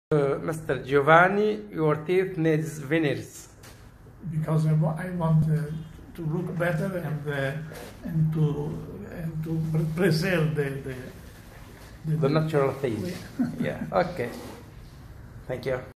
BEFORE VENEERS / From 🇮🇹 To 🇹🇳
Coming from Italy to Tunisia to have a perfect natural smile 😀

Italy Tunisia smile perfect veneers lumineers cad-cam#smiledeseign#drfitouri#